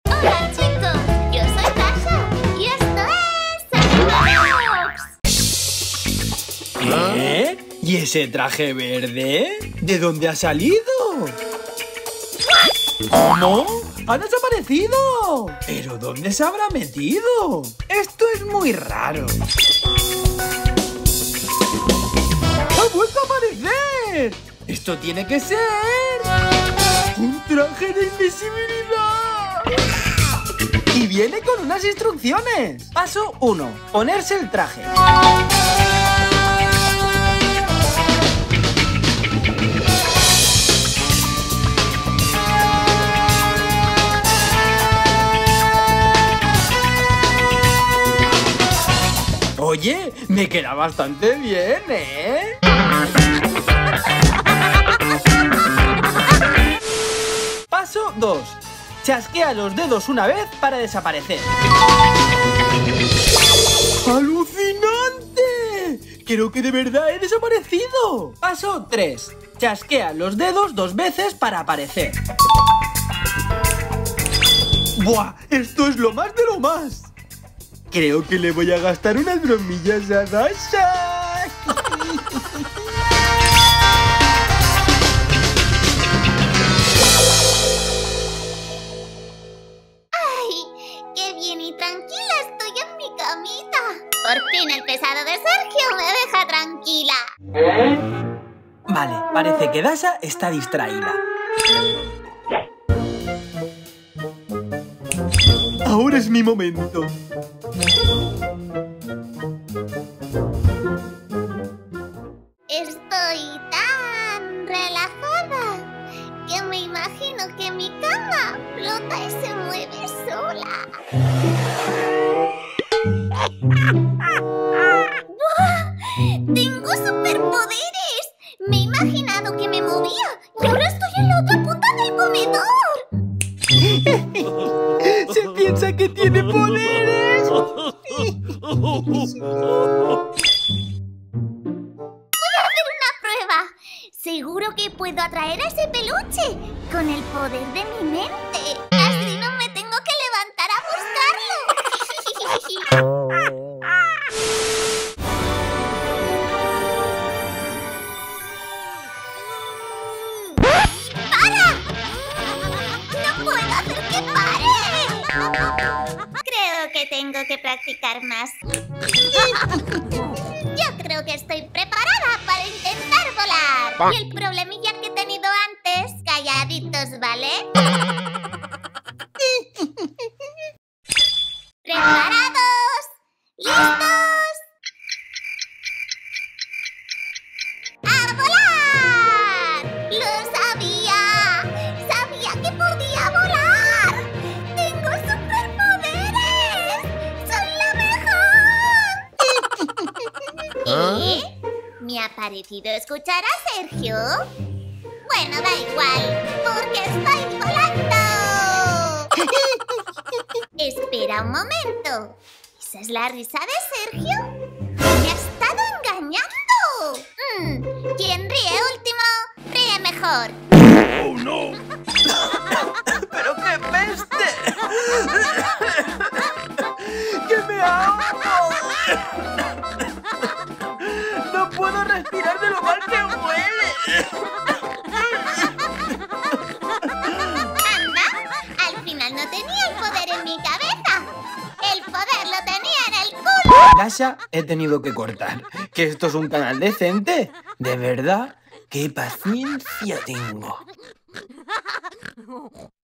Hombre Invisible ATACA a Perrita Dasha en la Vida Real / Vídeos de perros
Sergio encuentra un traje misterioso en casa de la perrita Dasha. Se lo pone y descubre que ese traje tiene el poder de volver INVISIBLE a todo el que se lo pone. ¡Sergio se convertirá en EL HOMBRE INVISIBLE en la vida real! En este capítulo Sergio vestido del hombre invisible gasta muchas bromas a la perrita Dasha. Mi perro no se entera de nada o eso creía yo... No te pierdas este nuevo capítulo de esta nueva serie con Sergio y Dasha y las mascotas más divertidas que hablan en español.